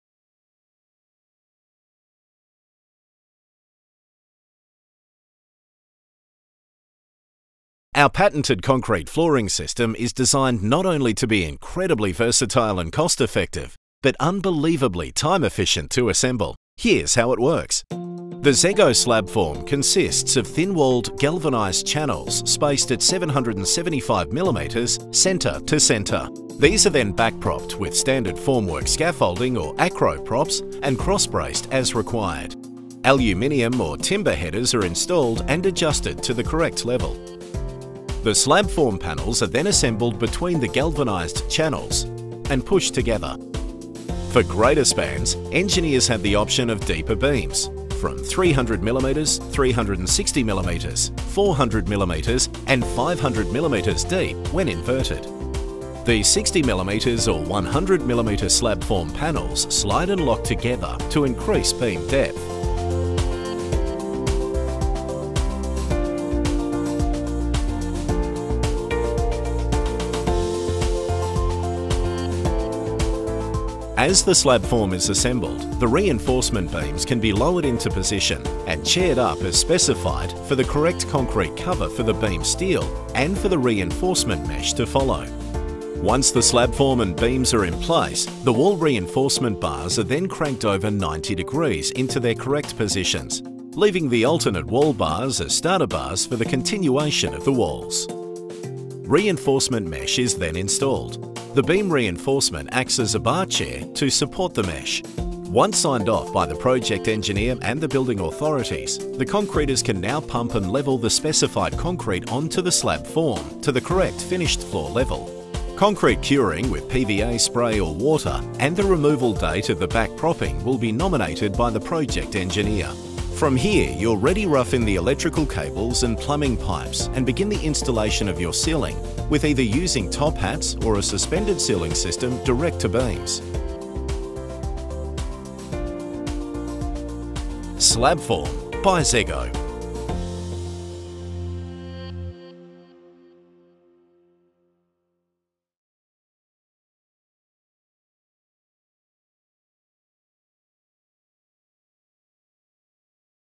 ZEGO ZlabFORM
ZEGO ZlabFORM is the newly introduced integrated and insulated, reinforced concrete suspended slab flooring system